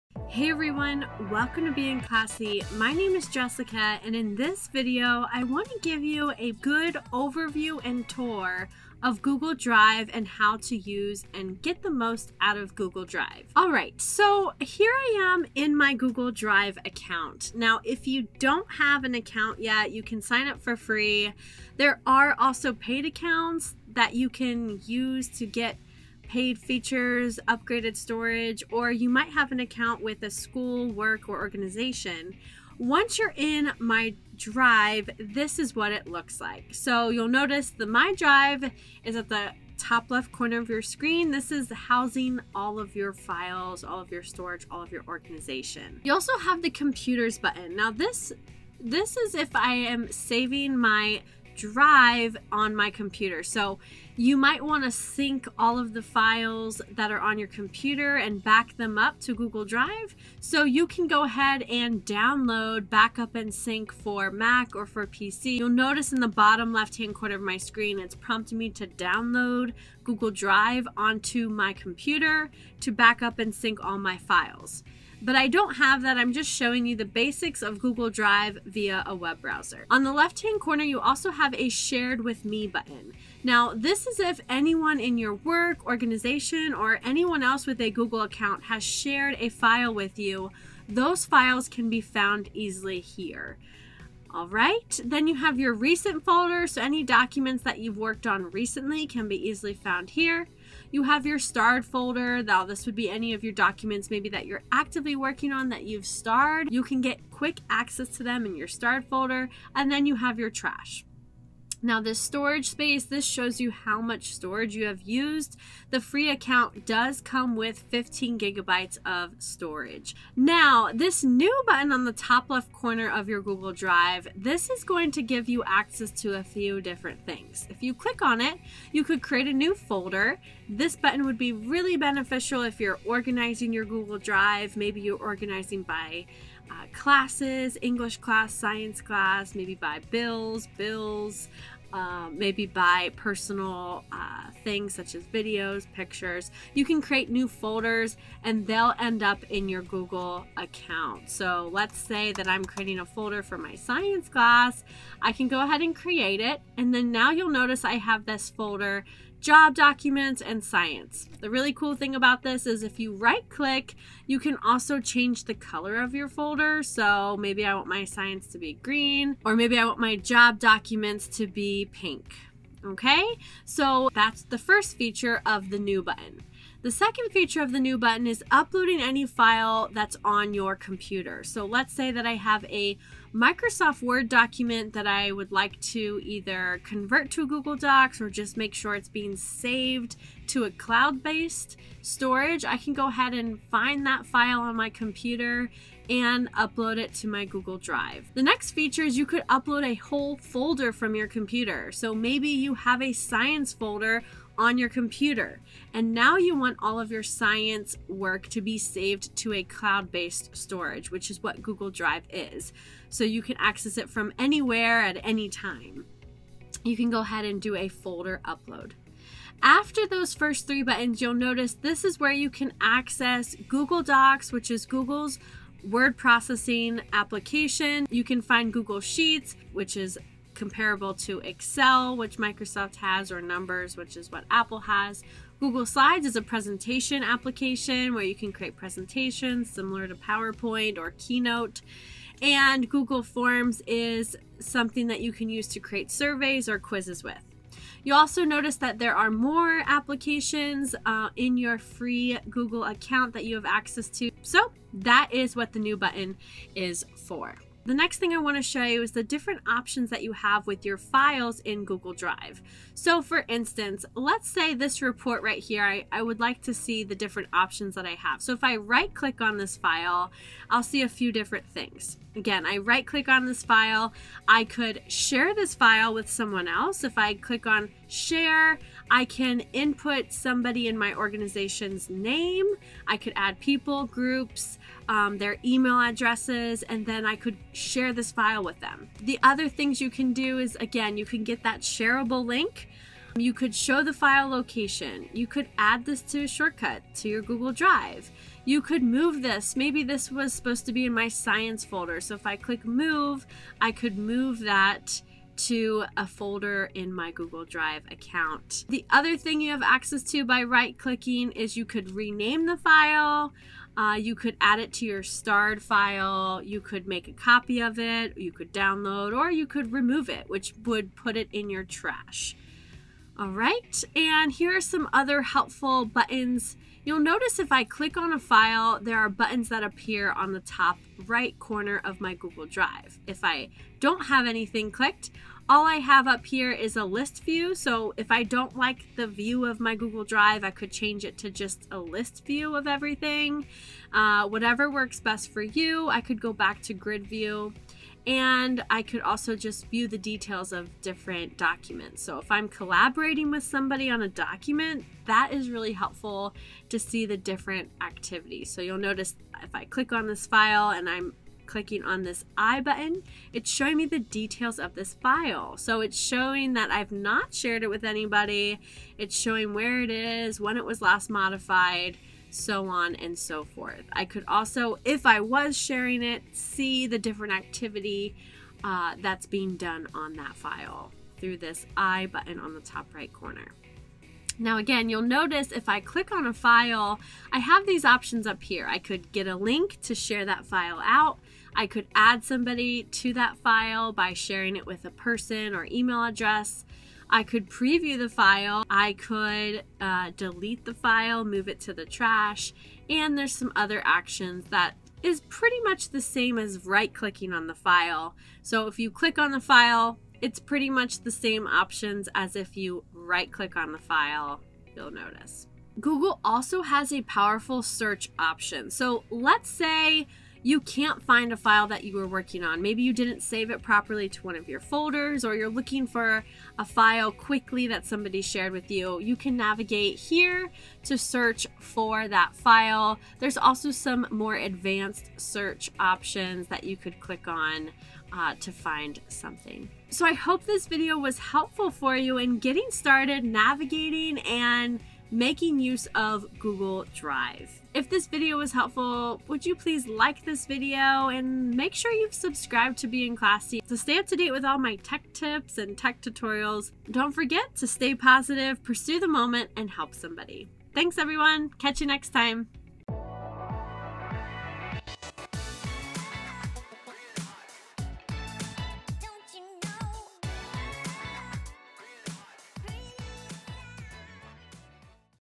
Getting Started with Google Drive - Tutorial for Beginners - How to Use Google Drive
In this tutorial, I explain what Google Drive is and show you the basics of how to get started using Google Drive.  Google Drive is a great storage and organization tool for school, work, and personal use.  In this video, I walk you through just the basic steps and features you need to know in order to get signed in and begin creating, saving, sharing, and organizing files in Google Drive.

Google Drive is a very useful tool that I use almost every day both in more work and at home. I truly hope you will find it useful as well!

Facebook: @beinclassy Want to show your support and gratitude for this video?